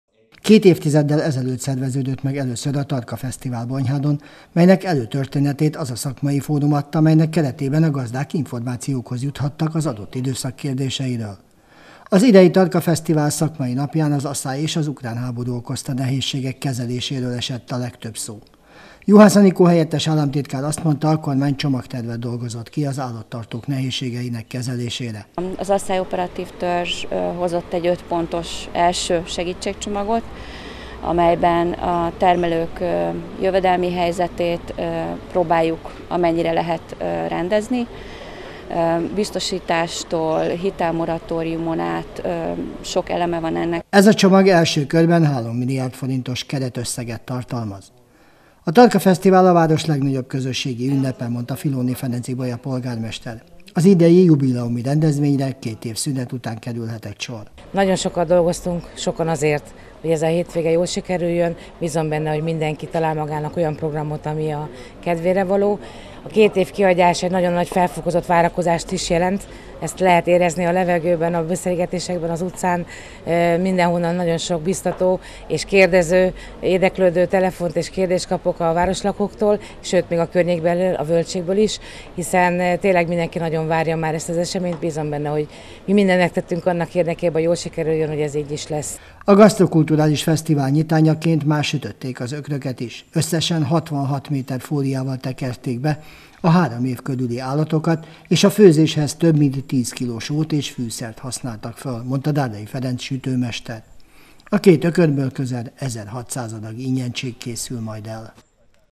Elkezdődött a 20. Tarka Marha Fesztivál - Bonyhád
Pénteken elkezdődött Bonyhádon a 20. Tarka Marha Fesztivál. A három napos programsorozat során gasztronómiai, kulturális és tudományos programok váltják majd egymást.